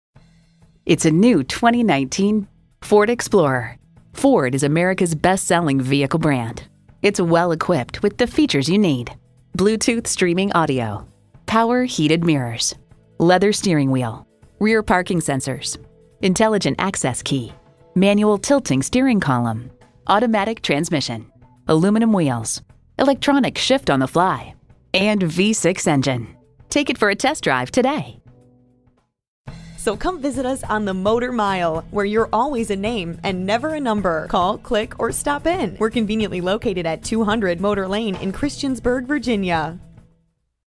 New 2019 Ford Explorer Christiansburg VA Blacksburg, VA FD190197 - SOLD
Year: 2019
Make: Ford
Model: Explorer
Trim: XLT 4WD
Engine: 3.5L V6 Cylinder Engine
Transmission: 6-Speed A/T
Color: UX INGOT SILVER METALLIC
Interior: 8W UNIQUE CLOTH BUCKET W/PWR DRVR EBONY BLACK INTE
Stock #: FD190197
VIN: 1FM5K8D86KGA69697